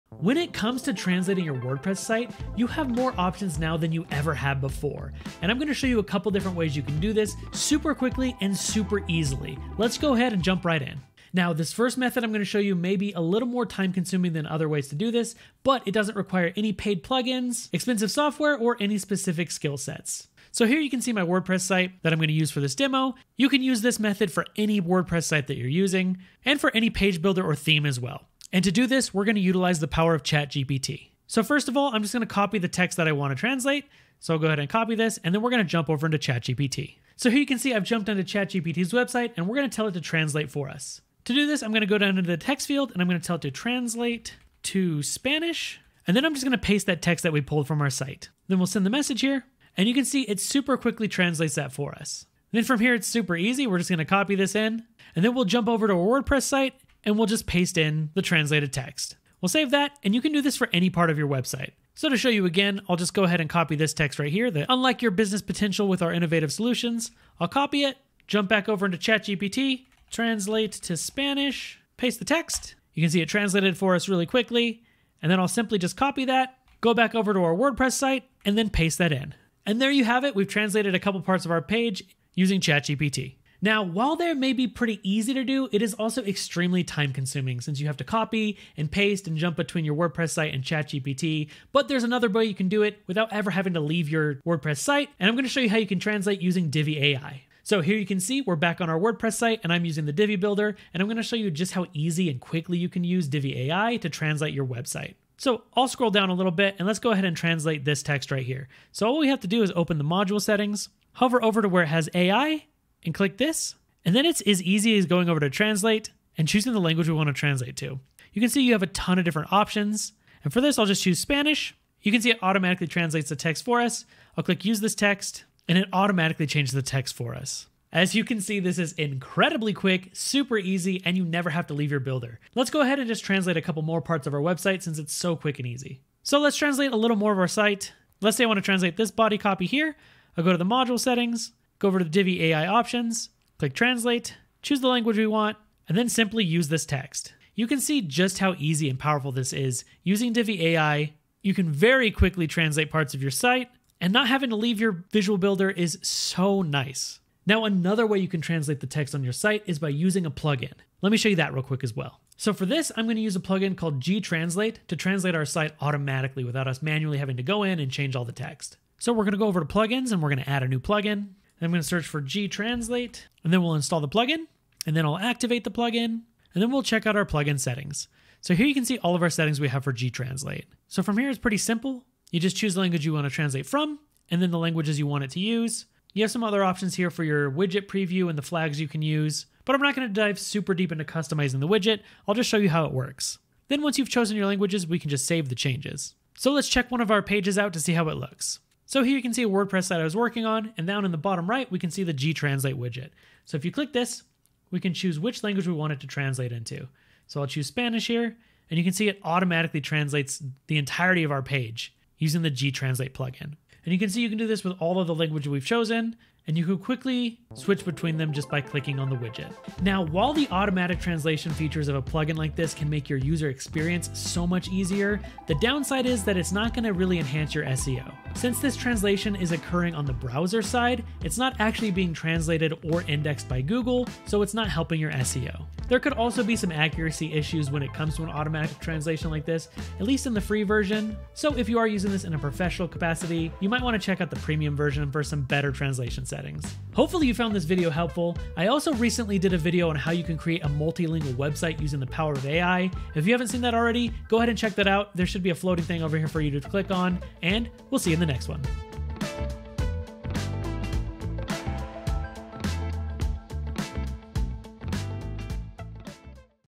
How to Translate Your WordPress Website with AI
Artificial intelligence (AI) can be a highly effective asset for translating your WordPress website. The AI language models that gave us ChatGPT (and its alternatives) and Google Translate are proving to be extremely useful for language translation more than ever before. Today we will show you How to Translate Your WordPress Website with AI!

Timestamps ⏰
0:00 Intro
0:10 Method 1
01:23 Method 2
02:37 Method 3
03:39 Final Thoughts